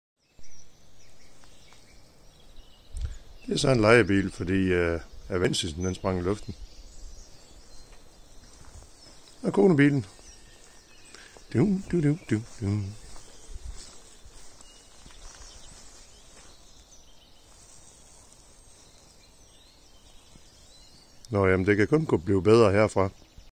Spontan rudesprængning
I går røg vandpumpen på min avensis. Det skiltes af i atomer. Der kommer en ny på mandag - så må vi se om motoren har nået at ta' skade.

Lejede en aygo og flyttede alt over. Den bette aygo var totalt proppet - sad så klemt at det vist ikke havde været nødvendigt at spænde sikkerhedsselen.

Kommer så ud her til morgen - bagruden i peugeot 107 er splintret spontant i løbet af natten. Go' start på påskeferien.